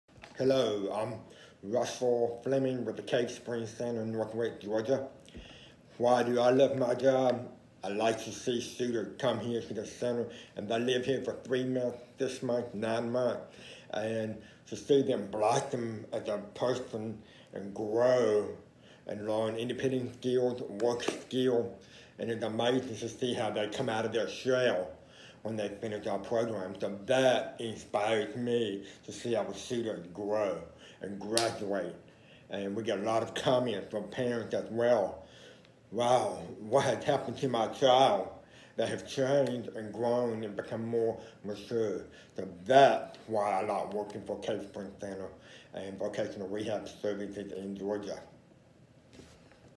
Russell Fleming, Senior Manager Cave Spring; VR Video Short
Russell Fleming describes why he loves VR and what motivates him.